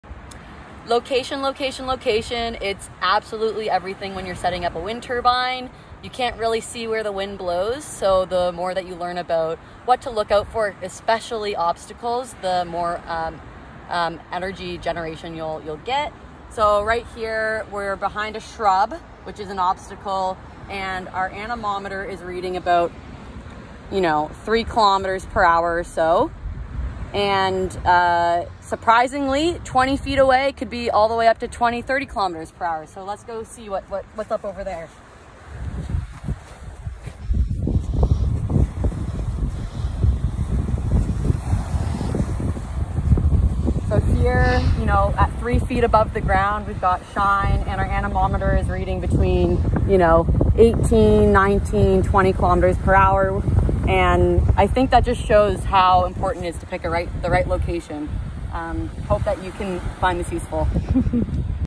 Choosing the best [windy] location for Shine
Location is key! 🔑 To optimize wind power generation, avoid setting Shine up by obstacles that can block wind flow like large bushes, trees, tents, or physical structures.

The best spots for Shine are wide open locations with laminar wind flow or a smooth consistent wind. This is especially important when Shine is operating at lower wind speeds.

Pro tip: We recommend using an anemometer to determine wind speeds when setting up Shine.